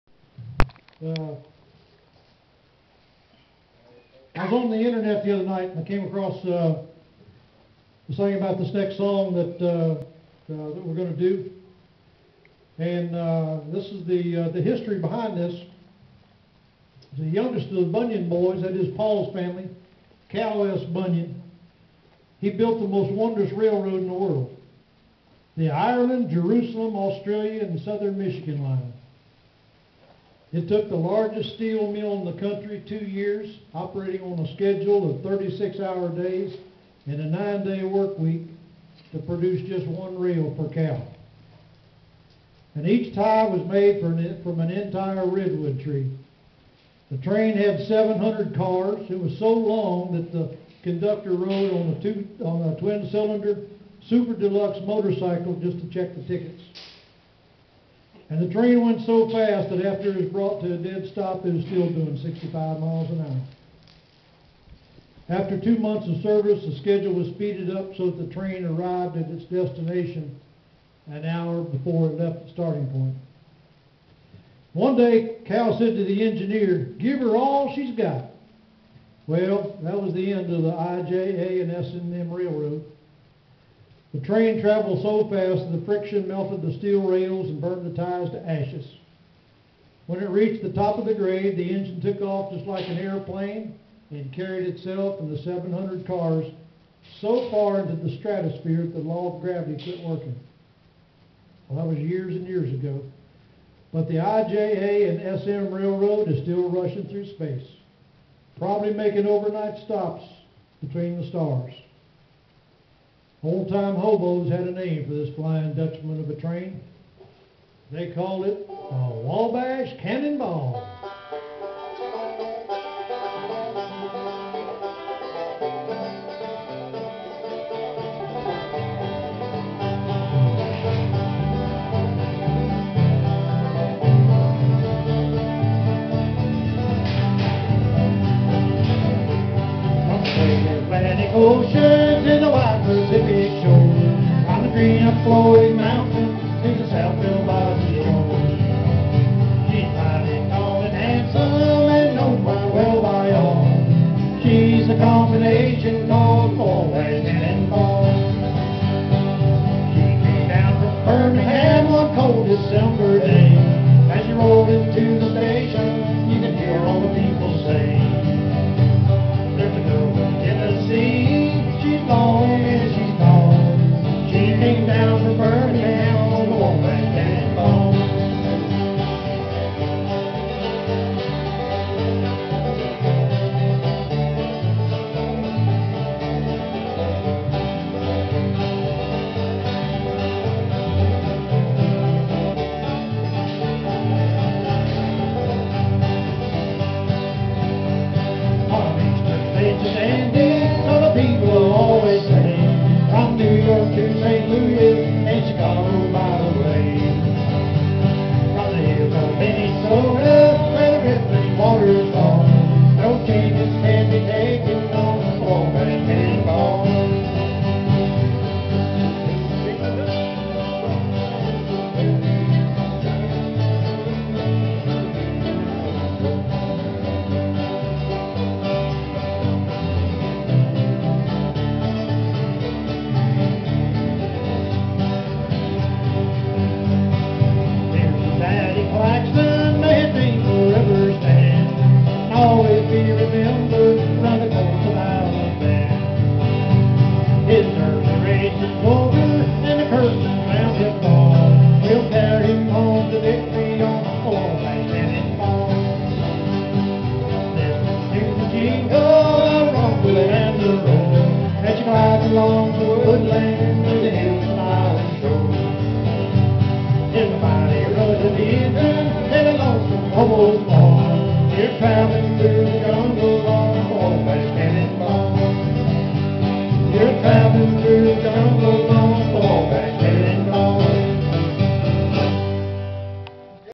A LITTLE HISTORY ON THE WABASH CANNONBALL
A handy little tidbit of information about one of the most famous, or infamous trains in all the world. THE WABASH CANNONBALL let'er go boys!!!